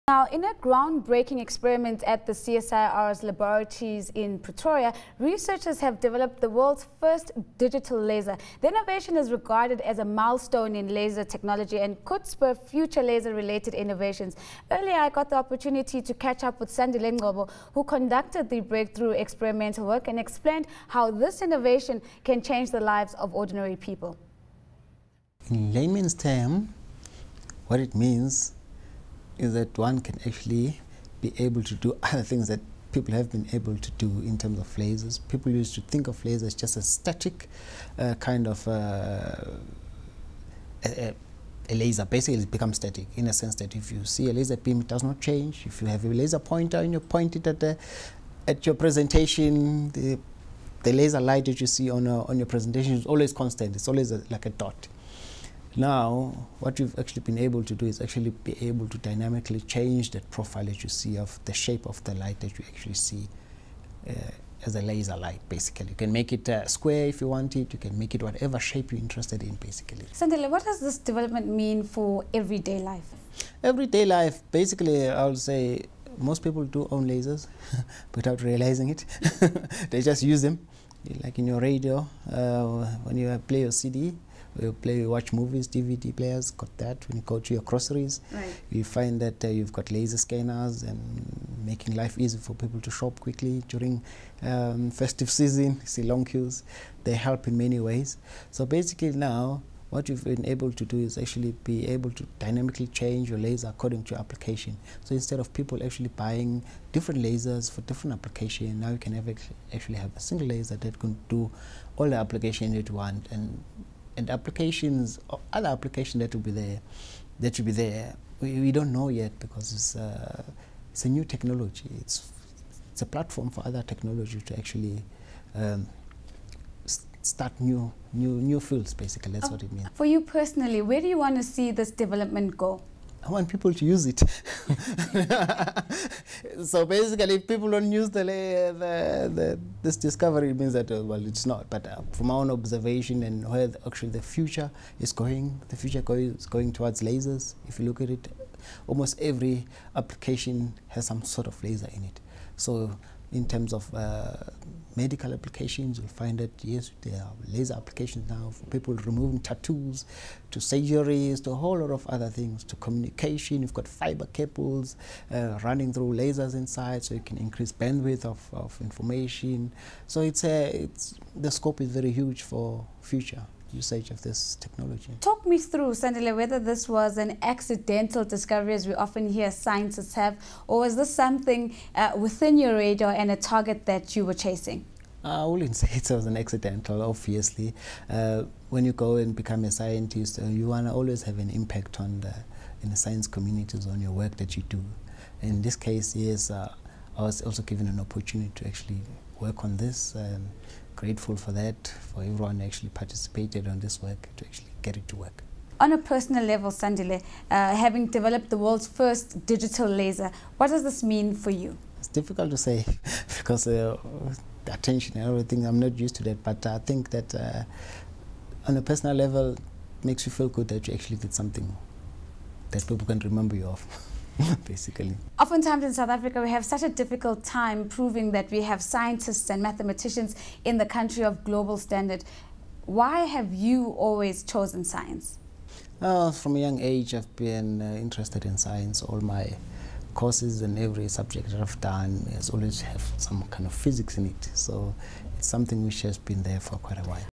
SA's CSIR develops world's first digital laser
Original Publish Date: || Thu, 19 Sep 2013 02:55:42 GMT || In a ground-breaking experiment at the CSIR's laboratories in Pretoria,  researchers have developed the world's first digital laser.